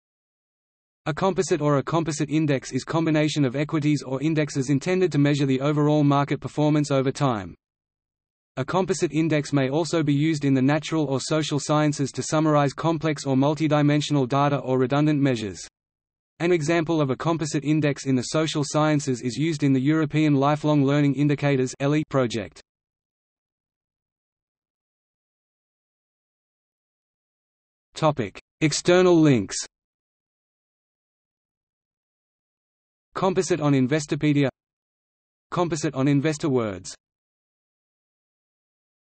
Composite (finance) | Wikipedia audio article
00:00:24 External links

- Socrates

SUMMARY
A composite or a composite index is combination of equities or indexes intended to measure the overall market performance over time.
A composite index may also be used in the natural or social sciences to summarize complex or multidimensional data or redundant measures. An example of a composite index in the social sciences is used in the European Lifelong Learning Indicators (ELLI) project.